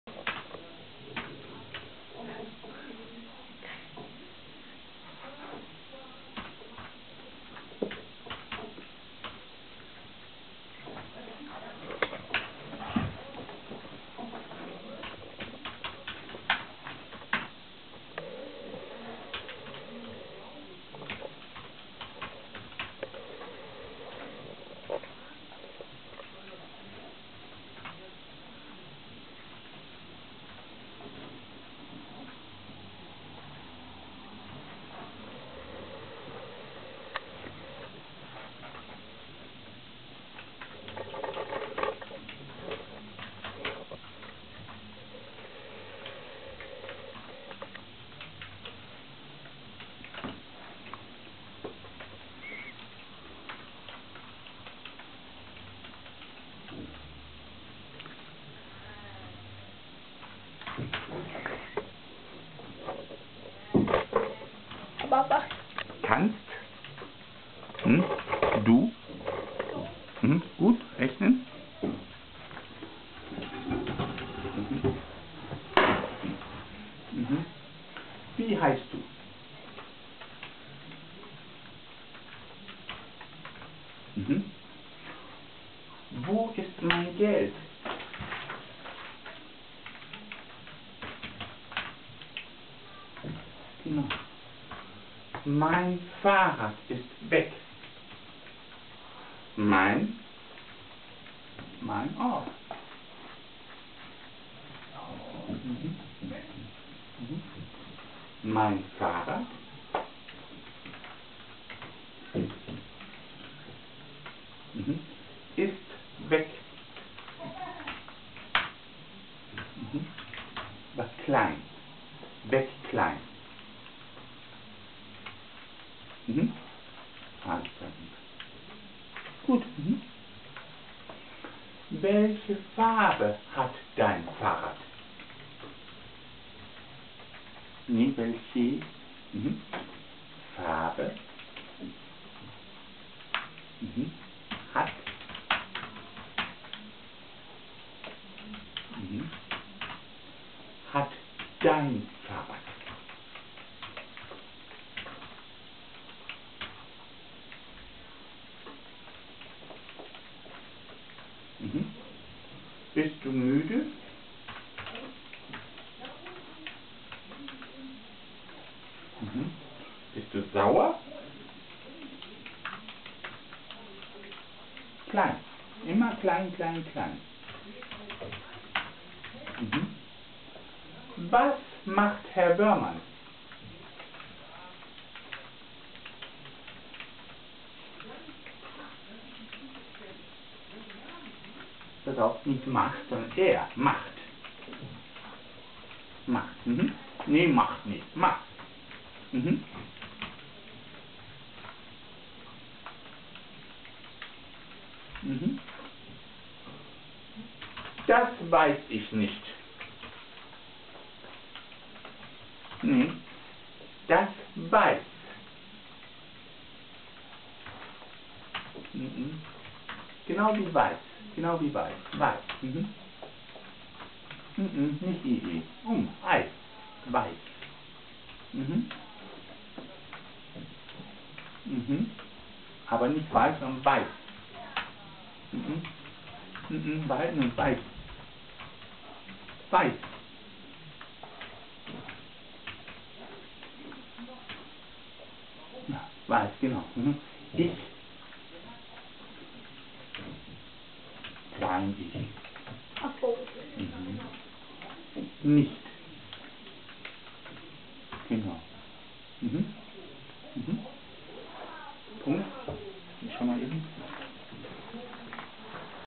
Wöhrmann´s SpeechWriting (Video 1)
SignWriting Symposium, Presentation 2

Teacher Stefan Wöhrmann works with his Deaf students at the Osnabruck School for the Deaf, using SignWriting and Wöhrmann's SpeechWriting – a way to write the movements of spoken language on the lips, using a newly-developed computer program called the DELEGS Editor.